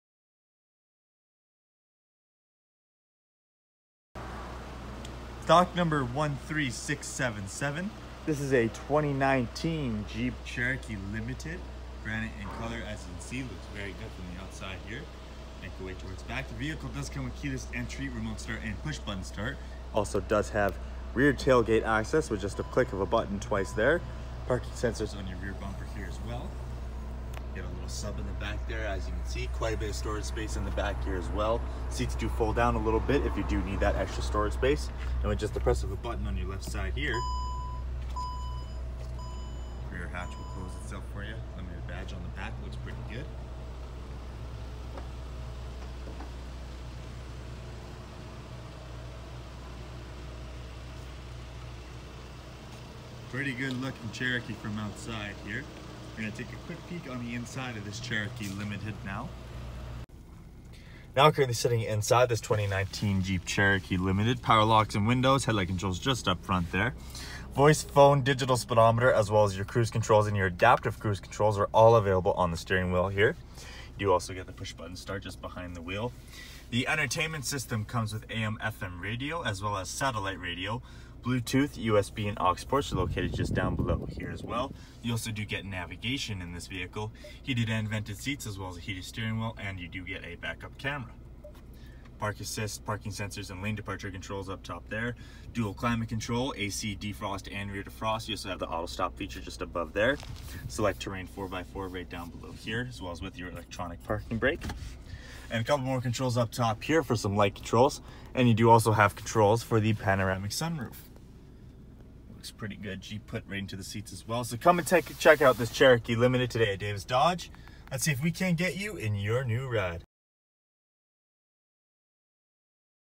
2019 Jeep Cherokee Limited
2019 Jeep Cherokee Limited walk through! At Davis Dodge in Fort Macleod